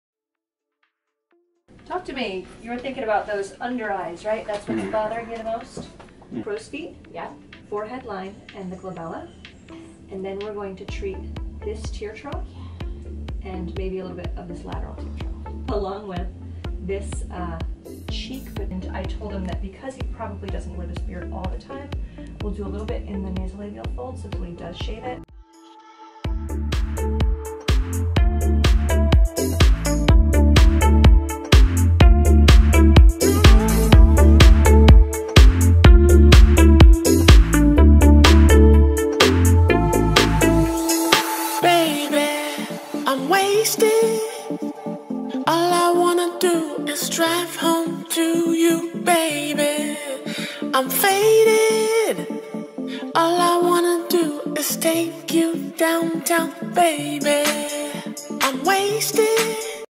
Male Pan-facial Rejuvenation using Restylane Lyft & Dysport
This guy's before & afters are incredible - he looks naturally 10 years younger! Here's a sneak peak into his consultation and treatment. He was treated with 3 syringes of Restylane Lyft throughout the medial cheek, tear trough, lateral tear trough, and nasal labial folds, as well as Dysport to the crows, glabella, and forehead. For bruise-less and painless treatment, his tear troughs and cheek were treated with Dermasculpt Microcannula.